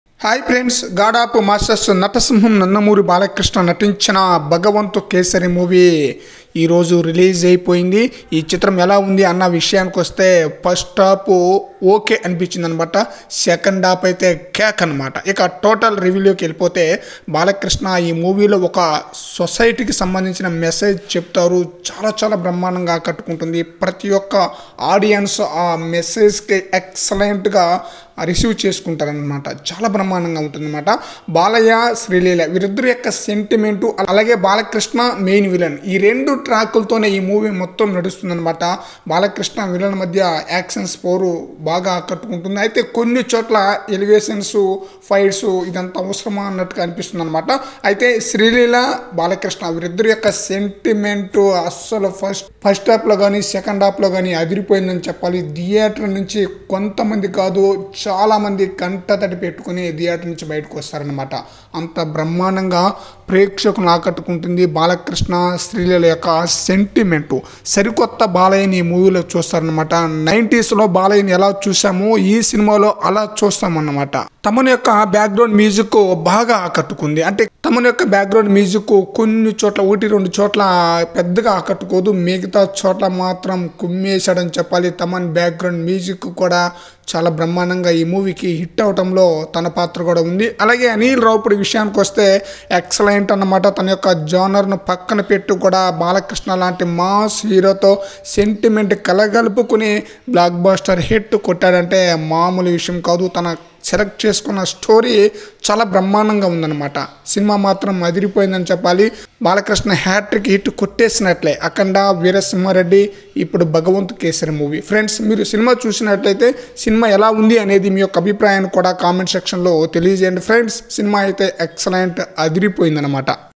Bhagavanth Kesari బొమ్మ హిట్టా ఫట్టా | Bhagavanth Kesari Review | Bhagavanth Kesari Public Talk
hi friends nandamuri balakrishna anil ravipudi combinationlo vachina bhagavanth kesari,bhagavanath kesari ee roju release ayindi,bhagavanth kesari review vishayaniki vaste cinema baga undi,balakrishna dialogues gani,sreeleela balakrishna sentiment baga work out ayindi
bhagavanth kesari review
bhagavanth kesari movie
balakrishna dialogues
balakrishna new movies
bhagavanth kesari public talk
bahgavaanth kesari diialogues
sreeleela